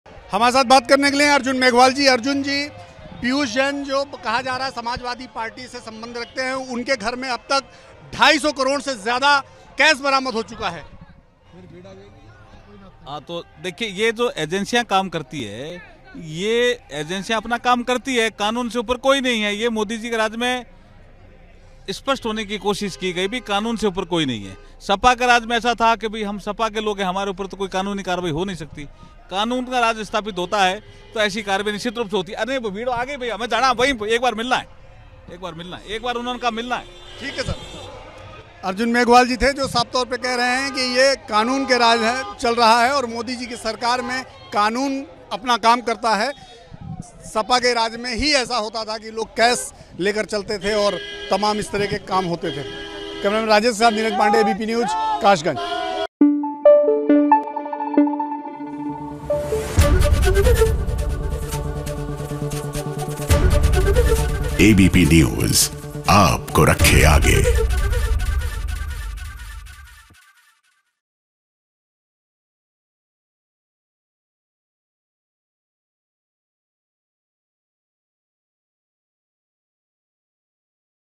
Kanpur Cash Case: Arjun Meghwal बोले- मोदी सरकार में कानून से ऊपर कोई नहीं, सपा के समय ऐसा नहीं था
कानपुर के कारोबाही पीयूष जैन के घर से अरबों रुपये कैश मिलने के मामले पर बीजेपी नेता अर्जुन मेघवाल ने कहा कि एजेंसियां अपना काम करती हैं. मोदी सरकार में कानून से ऊपर कोई नहीं है, सपा के समय में ऐसा नहीं था.